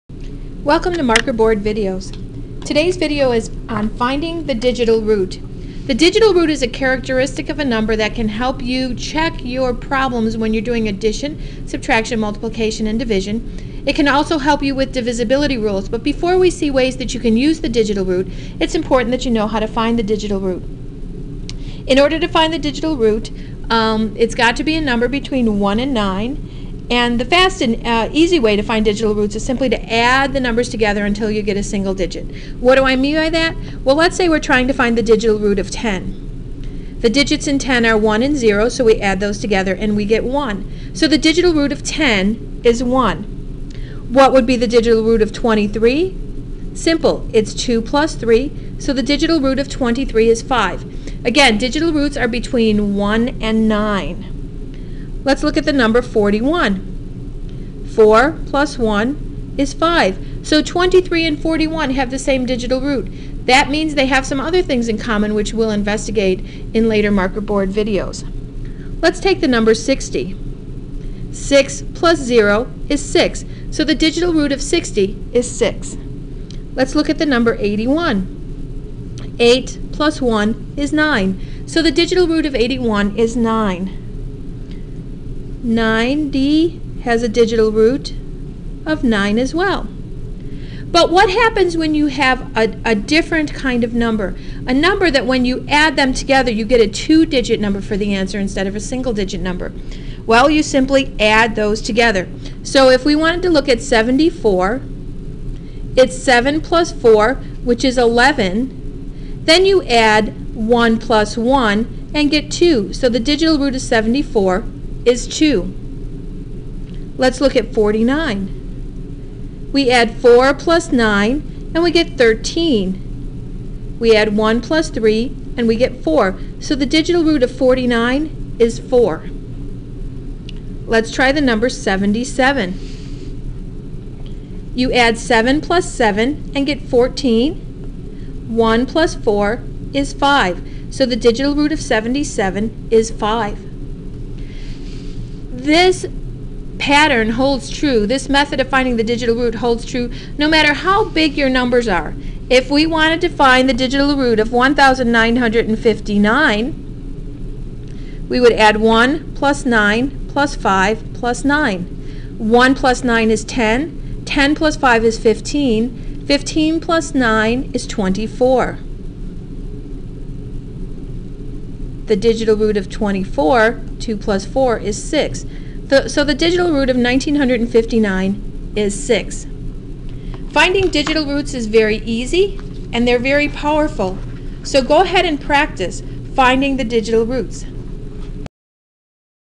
How to find a digital root
The digital root is a characteristic of a number that mades it easy to check addition, subtraction, multiplication and the divisibility of a number.